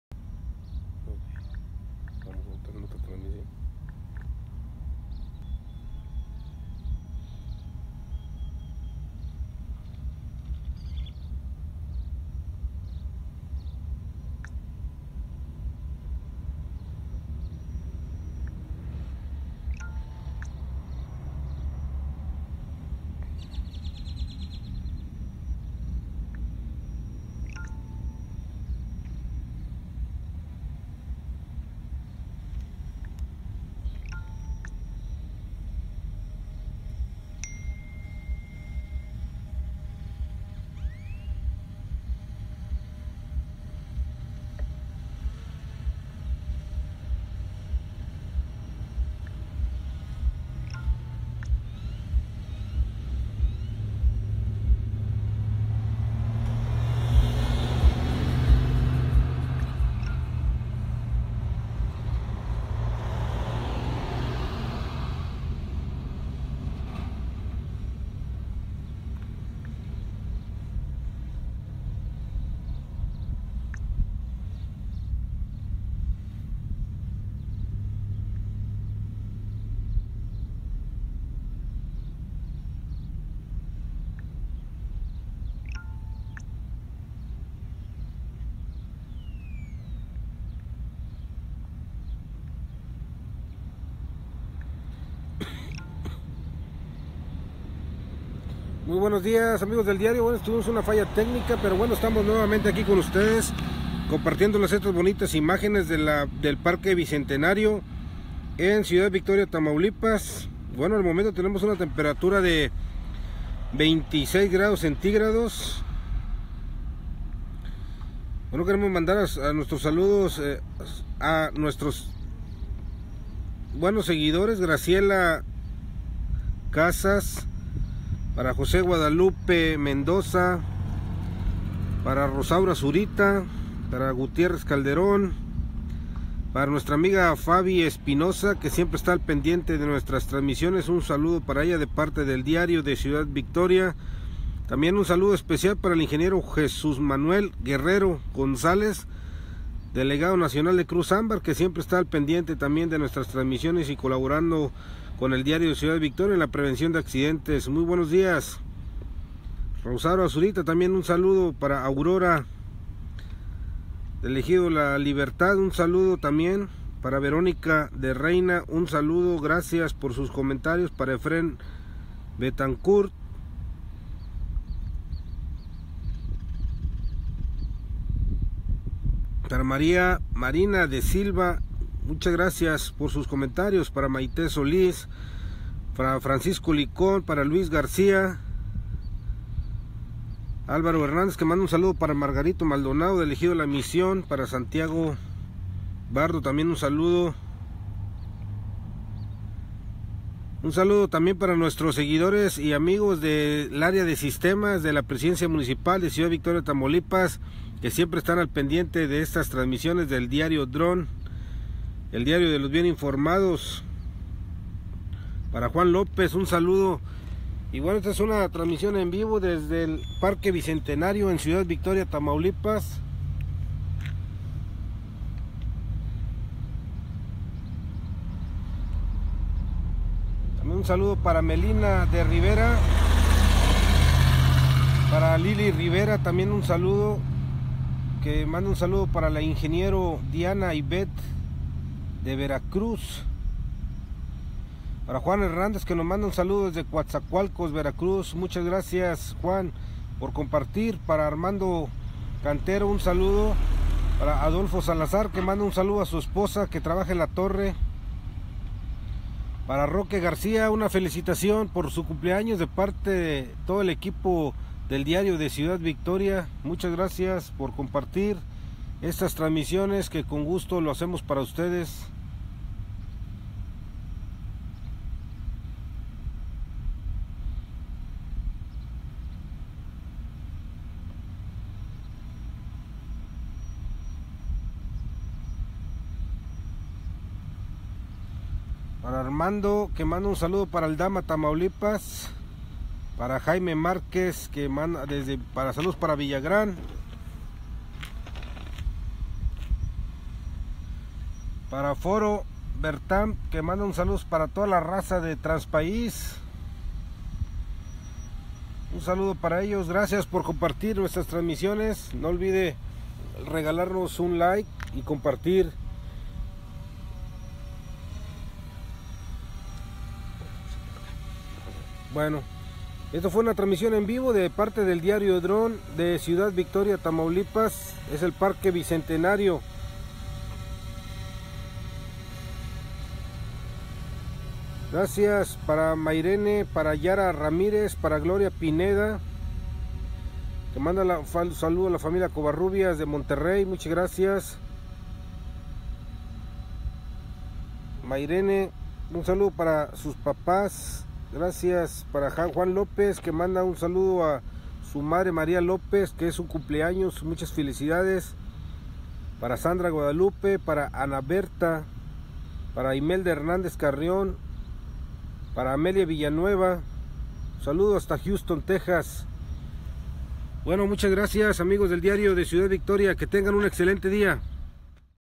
Te compartimos estas bellas imágenes desde el Parque Bicentenario
Te compartimos estas bellas imágenes desde el Parque Bicentenario, con imágenes de ElDiarioDron